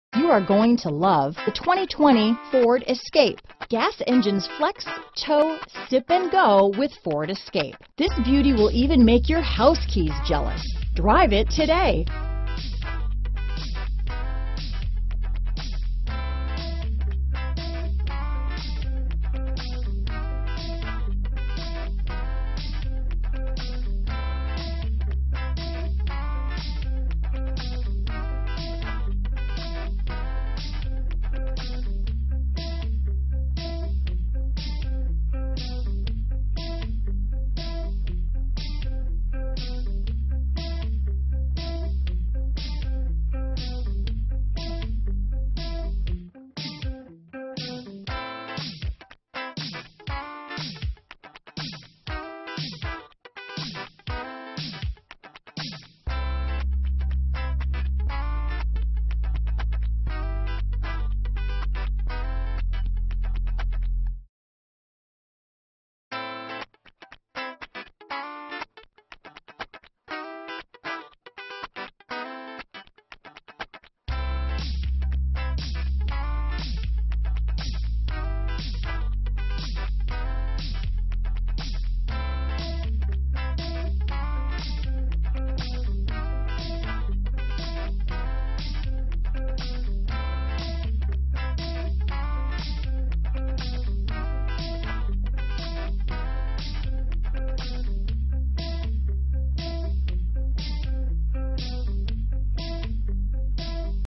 2020 Ford Escape Chantilly, Leesburg, Sterling, Manassas, Warrenton, VA C00412
New 2020 Ford Escape available in Chantilly, Virginia at Ted Britt Ford Lincoln. Servicing the Leesburg, Sterling, Manassas, Warrenton, VA area.
2020 Ford Escape Titanium AWD - Stock#: C00412 - VIN#: 1FMCU9J96LUA69593